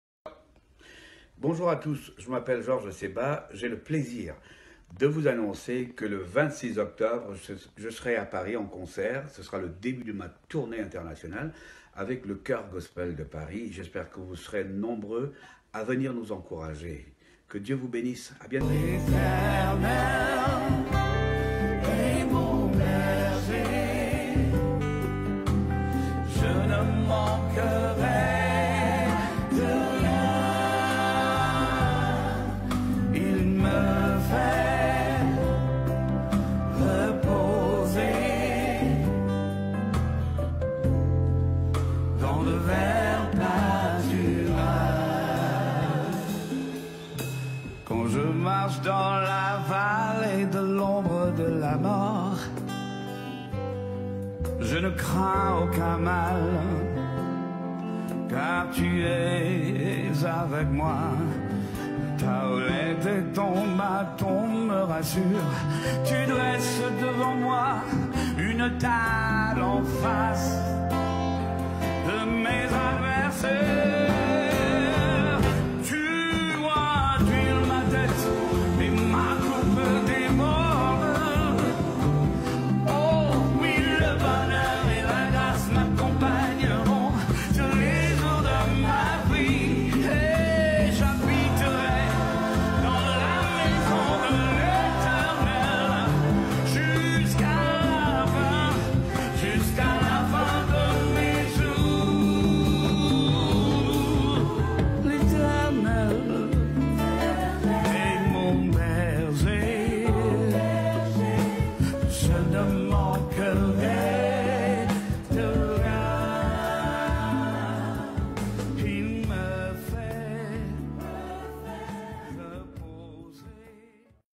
Georges Seba et sa nouvelle tournée
Bonjour à tous! Très bon weekend!
Je confirme notre présence ce 26 Octobre dès 20H à Paris.

Lieux : The American Church, 65 Quai D'Orsay, 75007 Paris
Tarif: 30€ (Adultes) 25€ (Prévente) et 12€ (Enfants - 15 ans)

Alors réservez déjà en ligne votre place sur ce lien